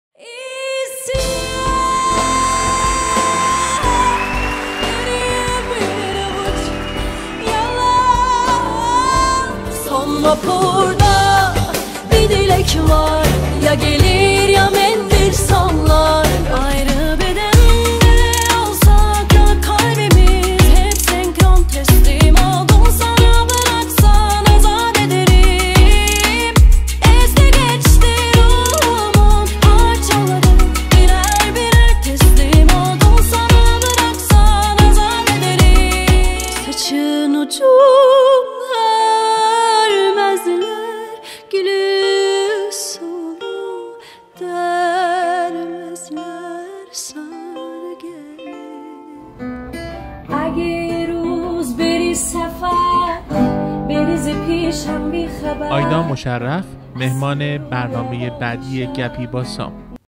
SamChats with Ayda Mosharraf (Teaser)
Ayda Mosharraf will join us for a chat about her latest artistic activities. If you have a question for Ayda go ahead and write it in the comments below!